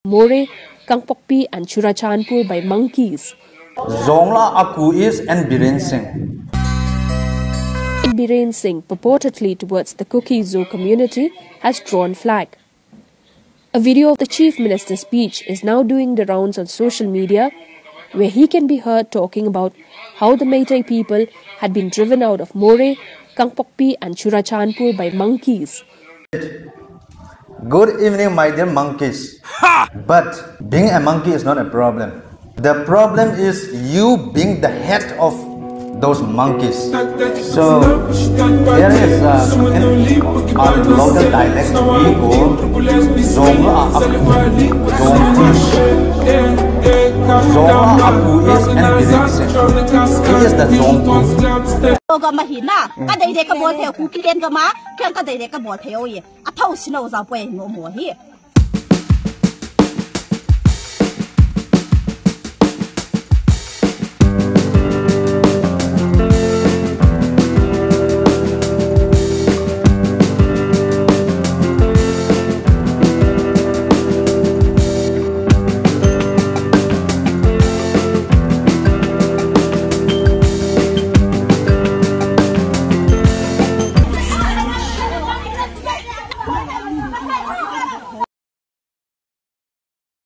N Biren Singh | Zong lah a jong aku 🤣| Zongku
N Biren Singh | Zong lah a hing aku 🤣| Zongku ‎@tungdymz

Manipur | A controversial remark made by Manipur Chief Minister N Biren Singh purportedly towards the Kuki-Zo community, has drawn flak. A video of the Chief Minister's speech is now doing the rounds on social media where he can be heard talking about how the Meitei people had been driven out of Moreh, Kangpokpi and Churachandpur by "Monkeys" impetuously referring to the Kuki-Zo community.

As per reports, Singh was addressing the concluding program of Nari Shakti Vandhan at the state BJP office on March 6.

Meanwhile, the Committee on Tribal Unity, or the CoTU, Sadar Hills expressed shock over the CM's remarks.